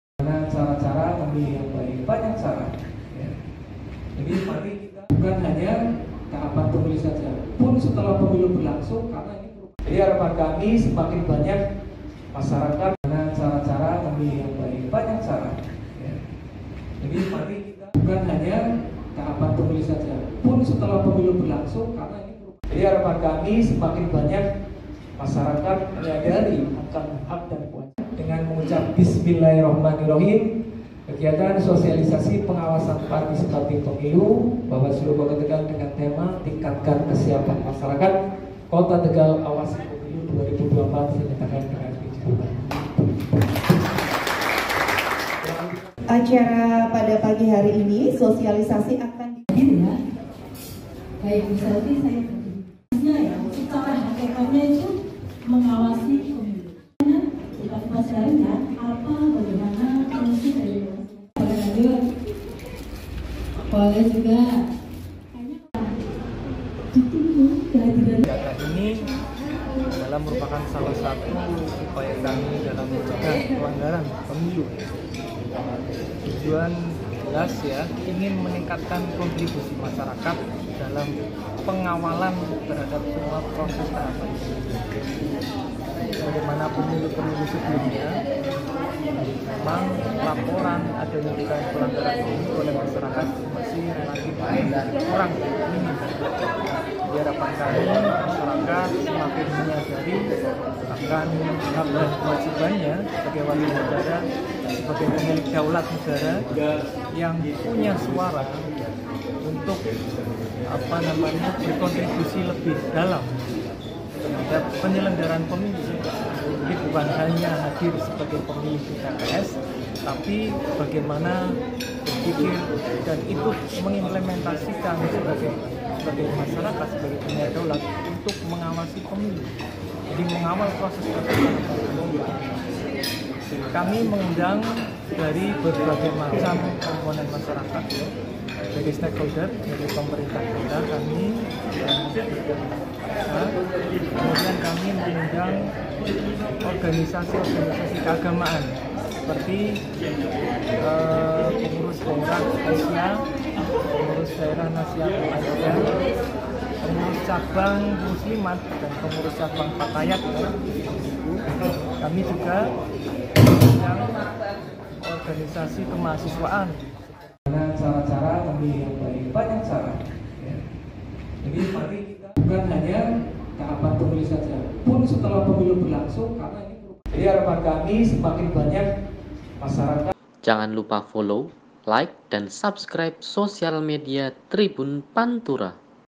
Bawaslu Kota Tegal Ajak Masyarakat Tidak Takut Awasi Pemilu 2024
Bawaslu Kota Tegal melakukan sosialisasi pengawasan partisipatif Pemilu 2024 dengan tema "Tingkatkan Kesiapan Masyarakat Kota Tegal
Awasi Pemilu 2024".

Devisi Penanganan Pelanggaran dan Penyelesaian Sengketa Bawaslu Kota Tegal, Nurbaeni mengatakan, masyarakat harus lebih aktif dalam mengawasi keberlangsungan Pemilu 2024.